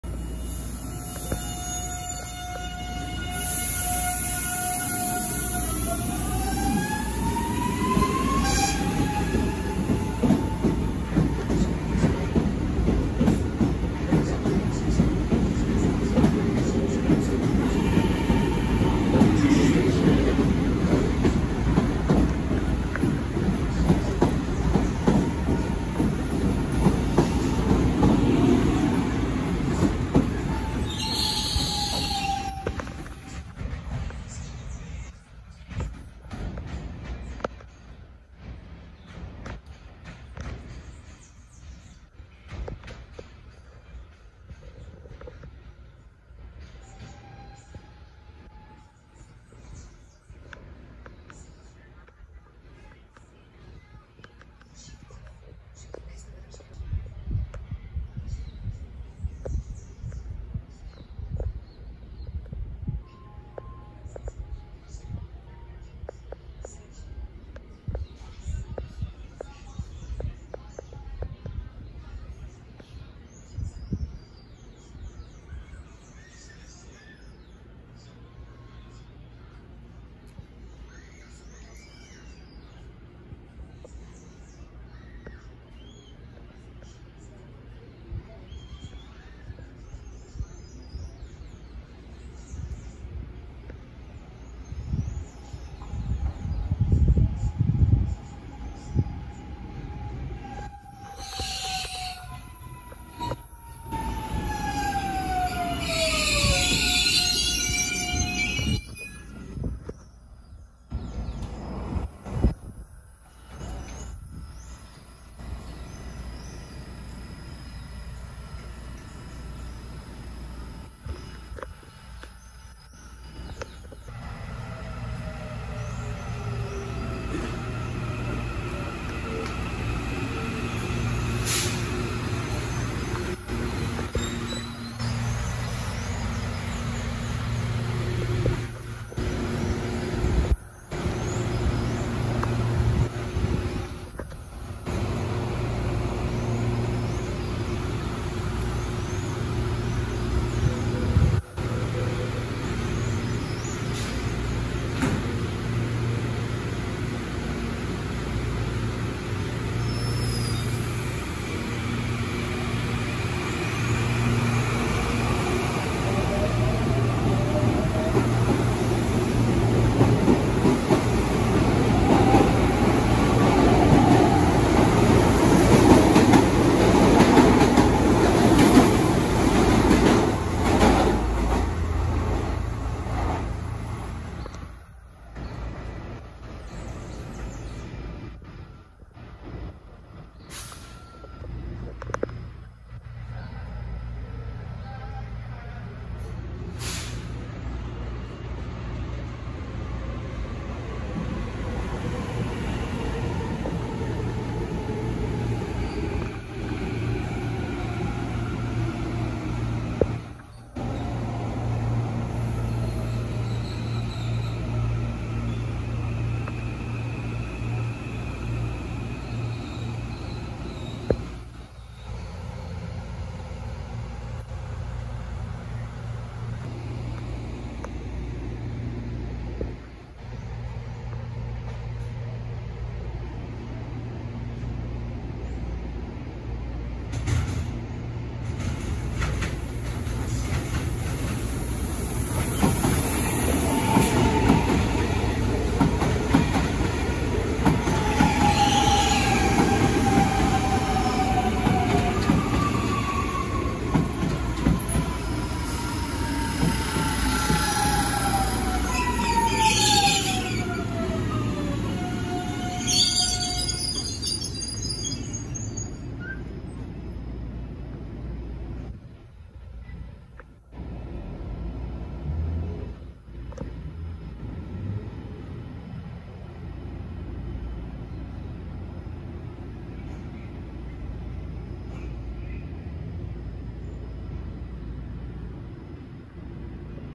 Trens na estação Presidente Altino - Linhas 8 e 9 | ViaMobilidade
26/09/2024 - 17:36

Gravação: Quinta-feira, 26 de setembro de 2024

Trens no vídeo:

Série A: A13 sentido Osasco - TUE {Alstom} | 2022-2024 (A131-A138)

A12 sentido Osasco - (A121-A128)

Série 7000: Q016 sentido Itapevi - TUE {CAF} | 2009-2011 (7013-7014/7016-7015)

Q020 sentido Barra Funda - (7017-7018/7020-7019)

A17 sentido Vila Natal - (A171-A178)